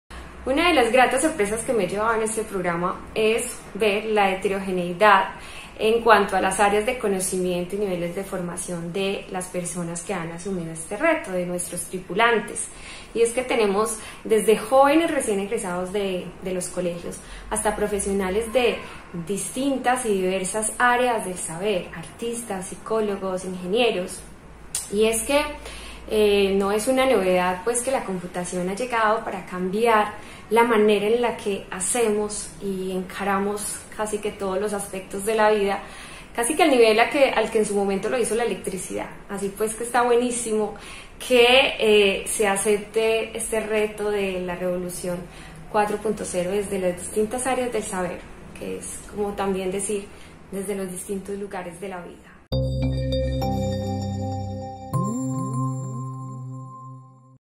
María Isabel Marín destaca el empeño de los tripulantes que se sumaron a las rutas de MisiónTIC2022
María Isabel Marín, docente de la Universidad de Antioquia, destaca la heterogeneidad en las áreas del conocimiento y los distintos niveles de formación de los tripulantes de MisiónTIC2022 que asumieron el reto de convertirse en expertos en computación.